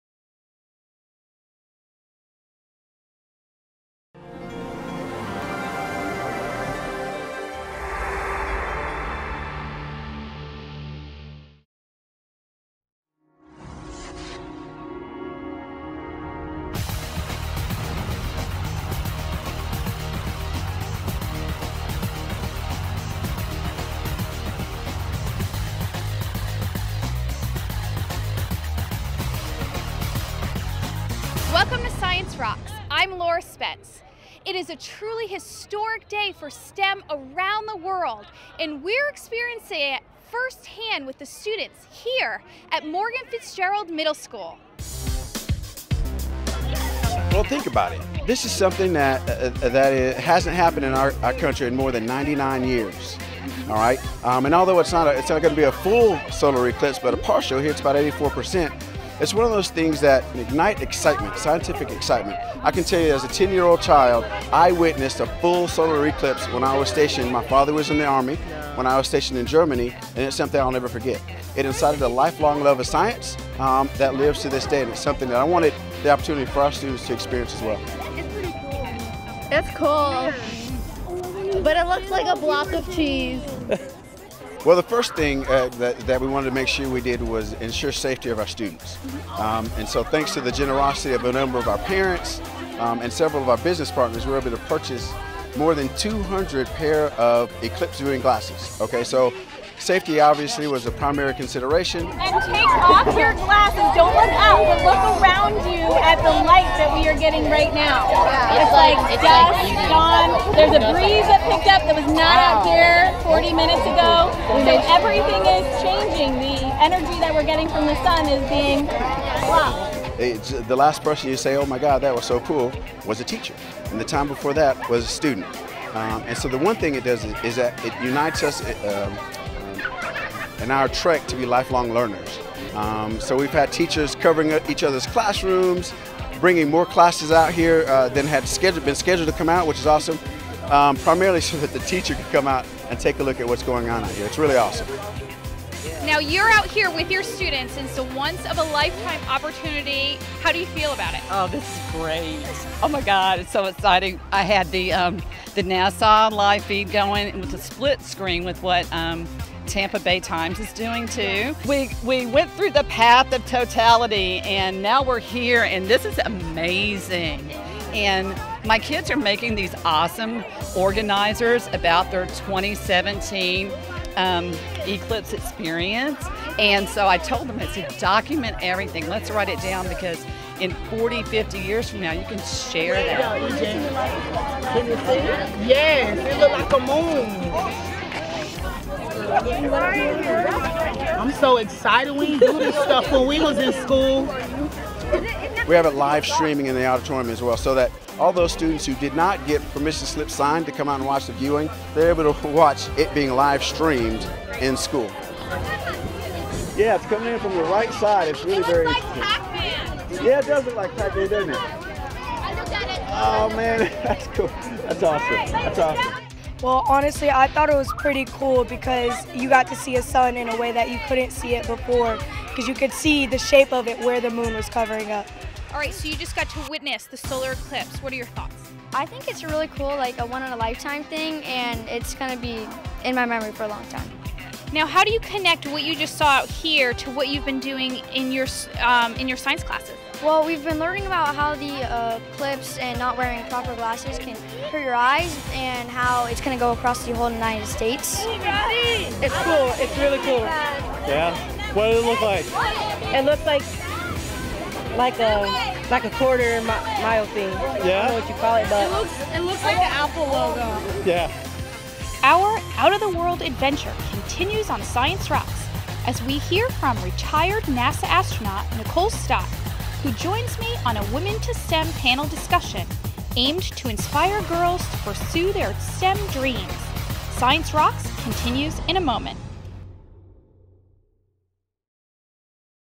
Science Rocks-November 2017-Solar Eclipse at Morgan Fitzgerald Middle School
In the November 2017 edition of Science Rocks, we experience the historic solar eclipse with students at Morgan Fitzgerald Middle School.